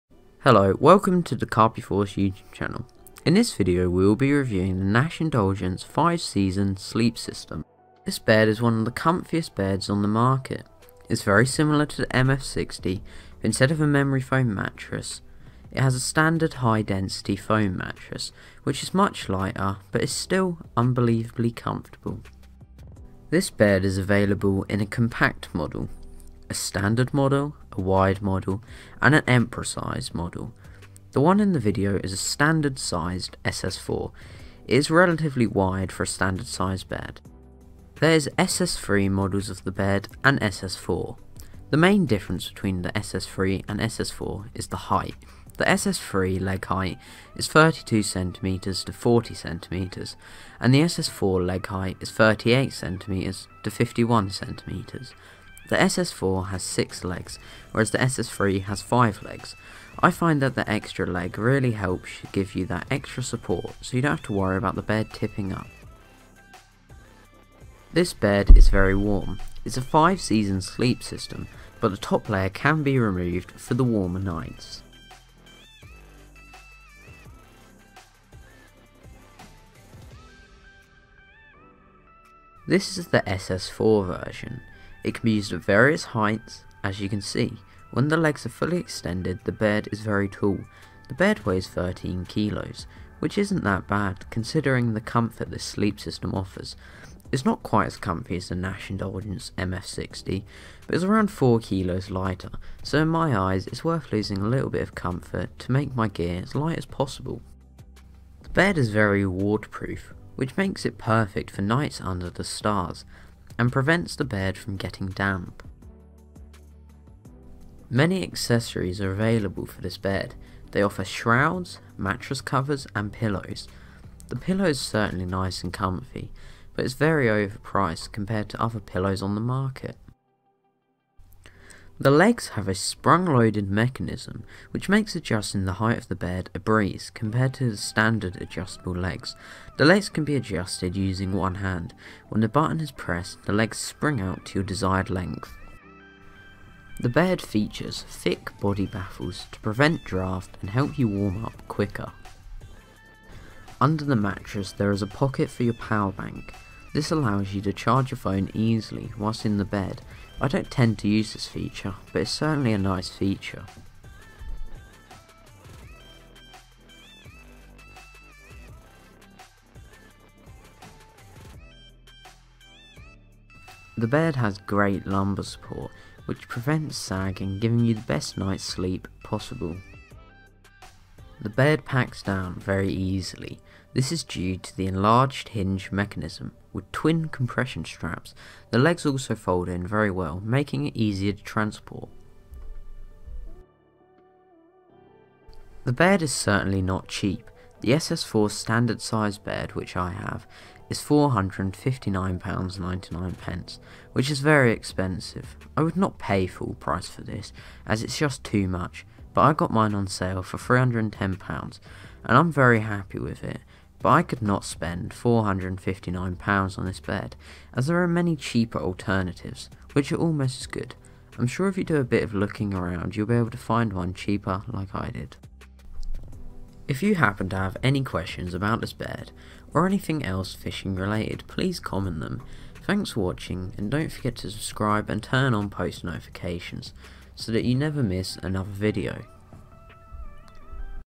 Nash Indulgence 5 Season Sleep System Review
Nash Indulgence Sleep System Review, in this video we review the Nash Indulgence 5 Season Sleep System Review, covering everything you need to know and show all the features. if you have any questions about this bed please do comment or DM us on Instagram. Thank you!

We are a carp fishing youtube channel, we post tutorials, vlogs, reviews, and much more!